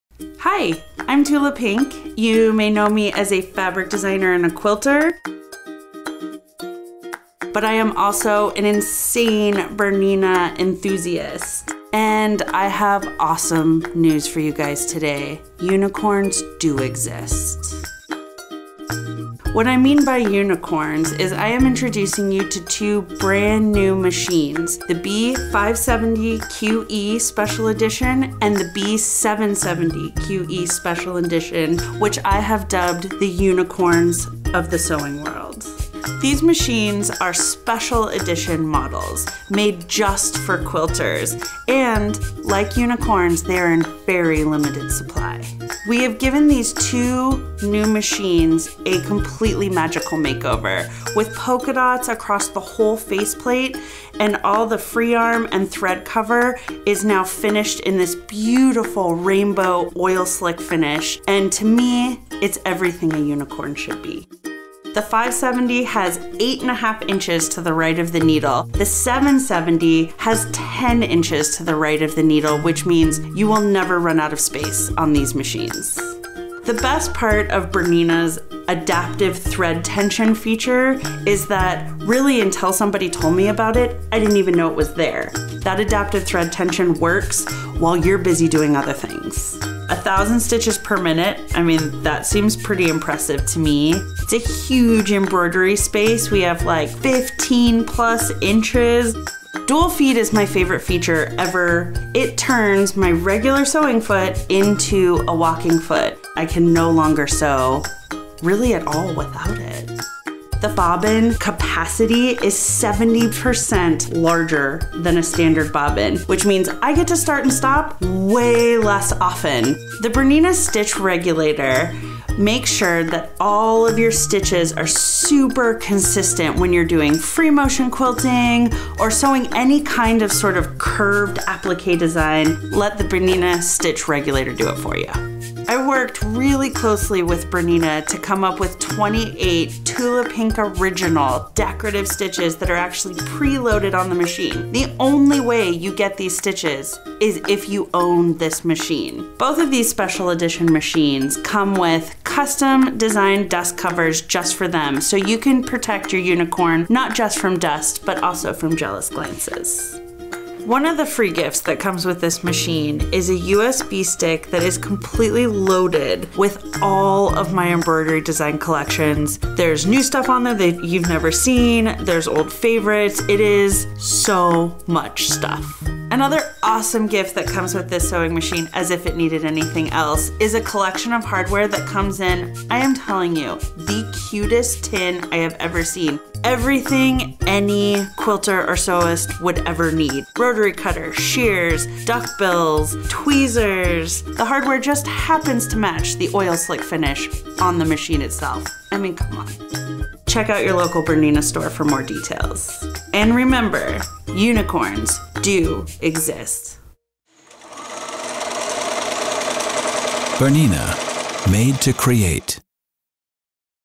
BERNINA Tula Pink Limited Edition Sewing Machine
Unicorn's do exist! This magically Sewlicious machine is coming to a BERNINA dealer in fall 2019. They are limited and they are awesome. Loaded with features, special stitches and goodies galore!